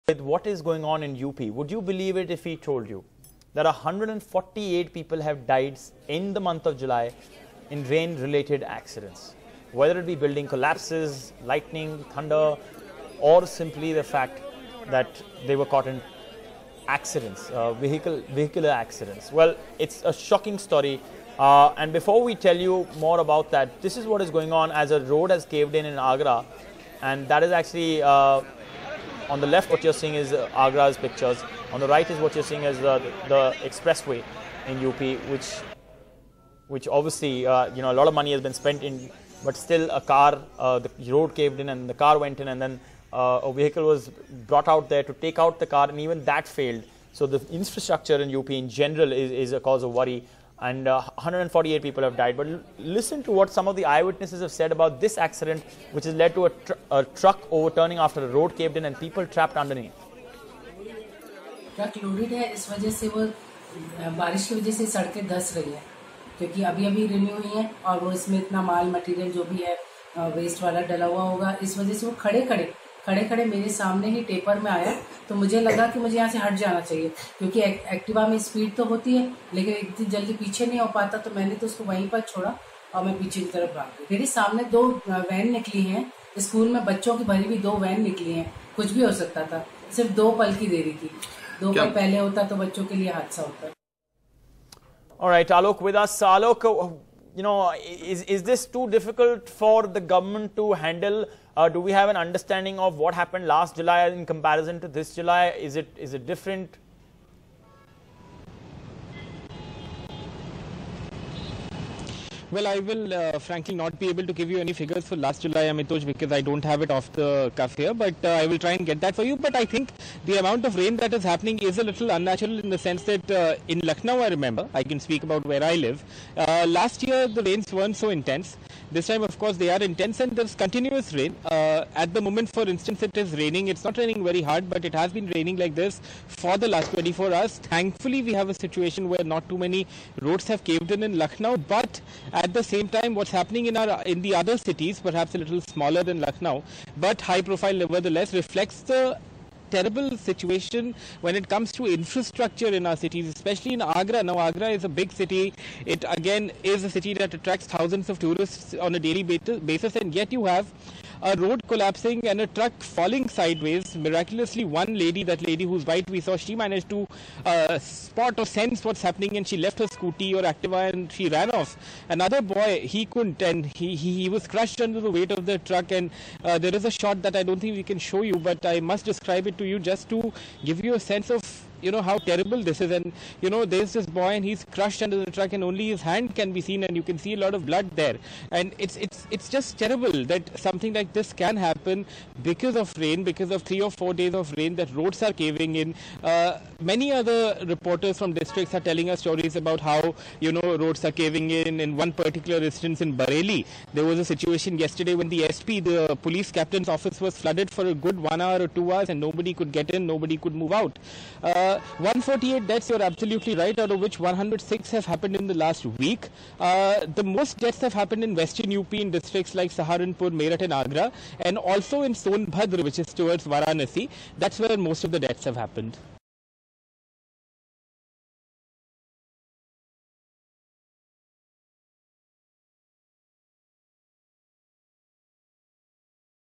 Man Crushed Under Truck, Woman Saved After Road Gives Away In Agra
A man was killed, but a woman had a miraculous escape in Uttar Pradesh's Agra when a truck fell after the road under it caved in this morning. Heavy rain in the city over the last week has led to the road cave-in, officials say.